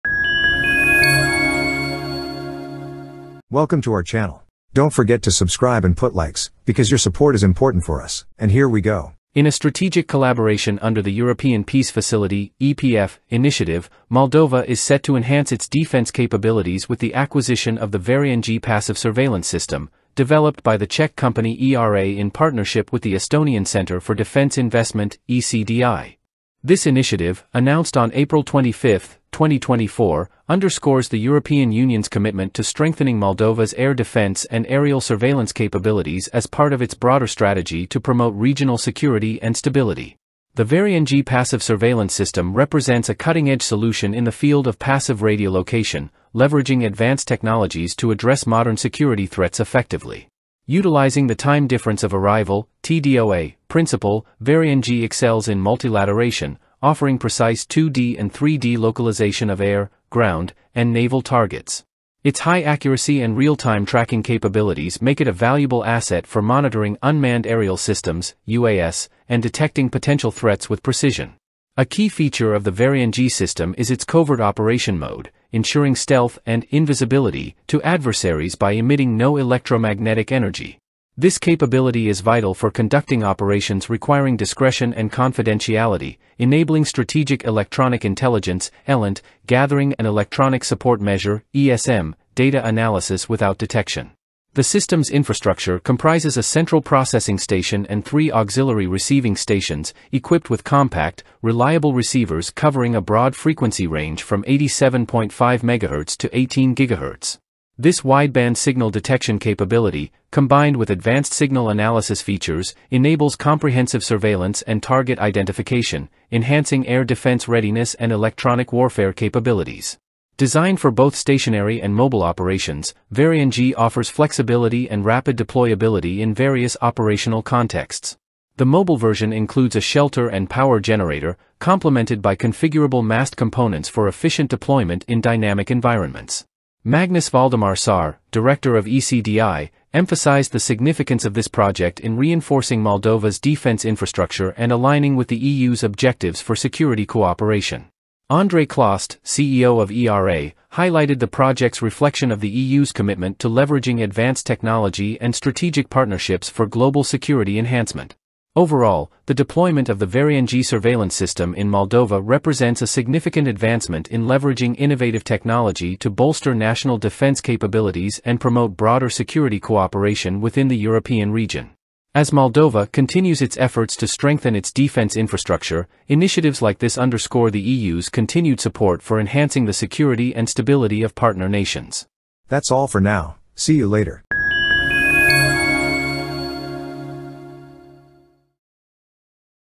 EU Aids Moldova in Defense Enhancement with VERA NG Surveillance Systems
In a strategic collaboration under the European Peace Facility (EPF) initiative, Moldova is set to enhance its defense capabilities with the acquisition of the VERA-NG Passive Surveillance System, developed by the Czech company ERA in partnership with the Estonian Centre for Defence Investment (ECDI). This initiative, announced on April 25, 2024, underscores the European Union's commitment to strengthening Moldova's air defense and aerial surveillance capabilities as part of its broader strategy to promote regional security and stability.